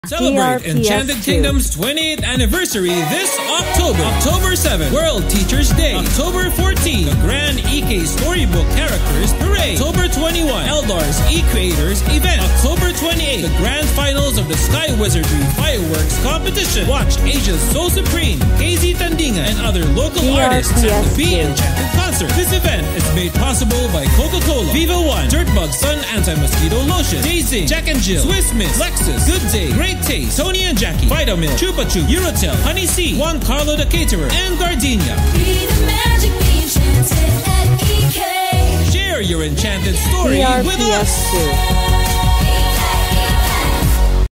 Enchanted Kingdom 28th Anniversary 2023 Events Radio Ad October 2023 44s
This is the October 2023 Radio Ad for Enchanted Kingdom, on what's happening this 28th Anniversary of this ammusement park.

DRPS Two 5366: Enchanted Kingdom 28th Anniversary 2023 Events Radio Ad October 2023 44s